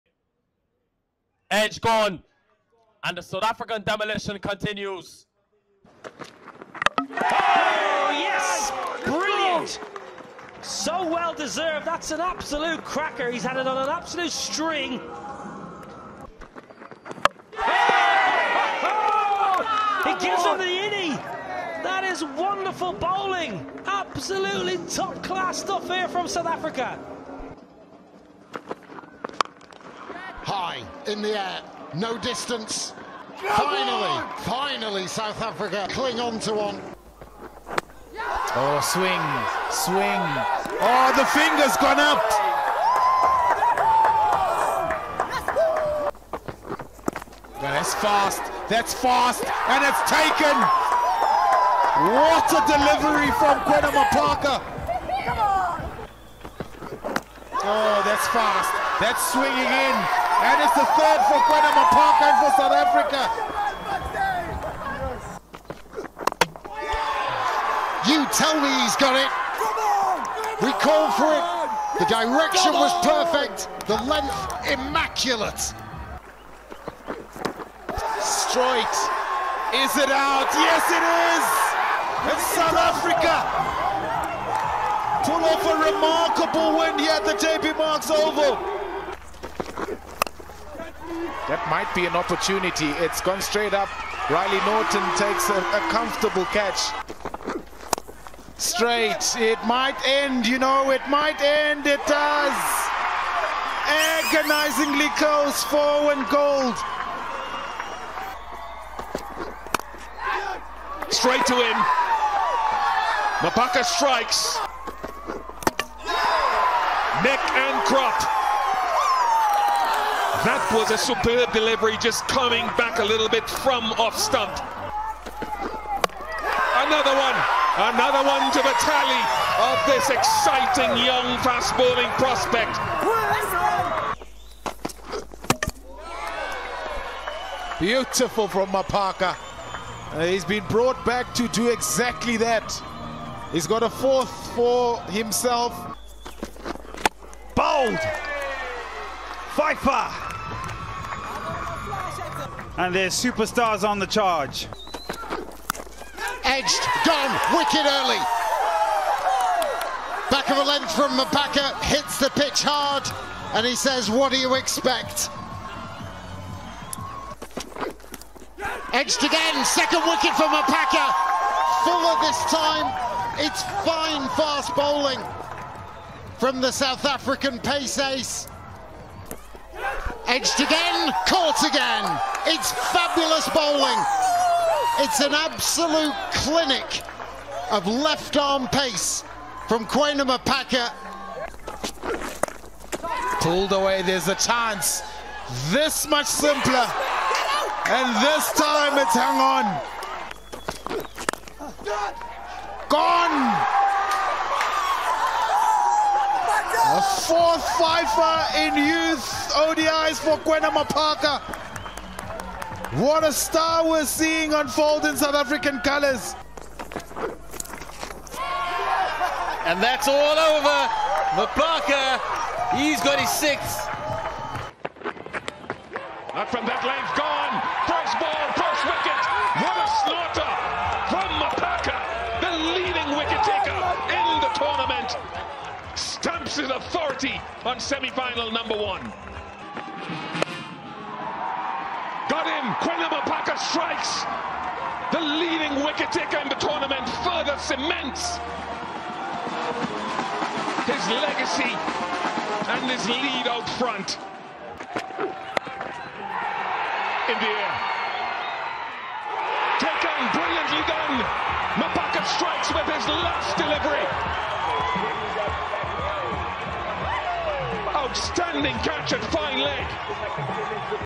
The unstoppable Kwena Maphaka | U19 World Cup wicket compilation
South Africa bowling sensation Kwena Maphaka equalled the record for most wickets in ICC U19 Men's Cricket World Cup history with 28 wickets across the two editions he has featured. Maphaka also became the first player to take three five-wicket hauls in a single event.

Maphaka took 7 wickets in the 2022 U19 World Cup and finished with 21 in the 2024 edition.